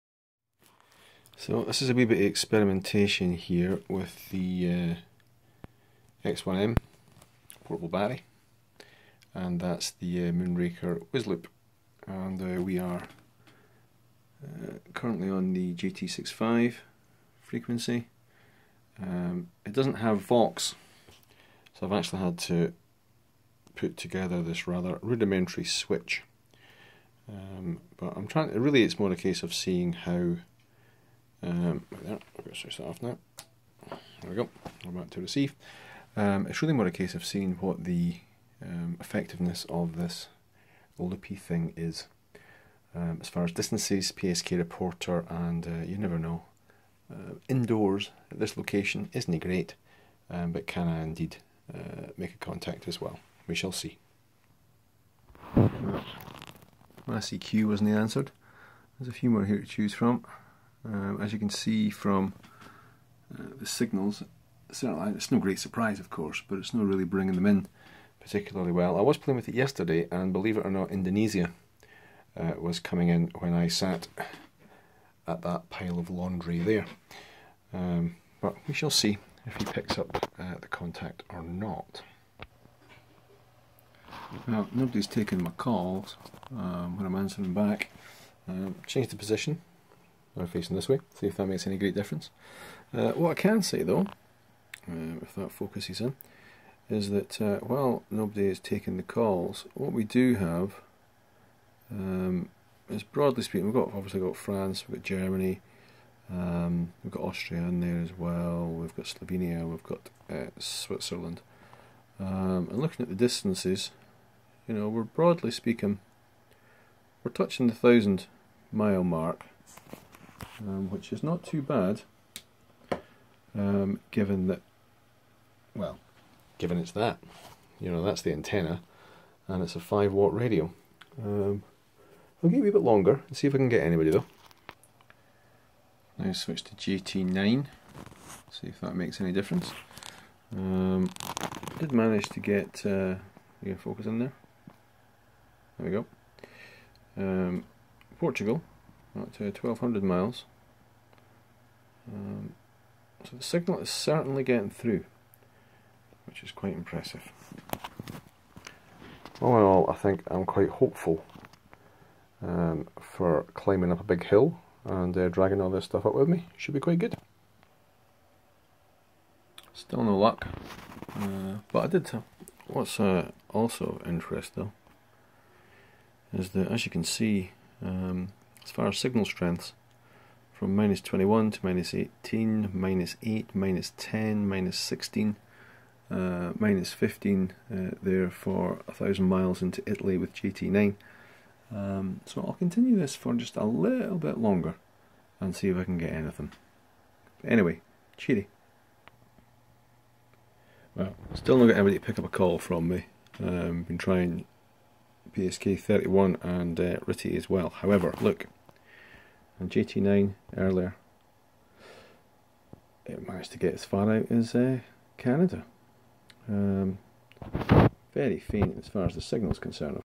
Moonraker Whizz Loop - X1M QRP rig. Indoors. Signal tests.
As it says. Does the wee antenna actually send anything out.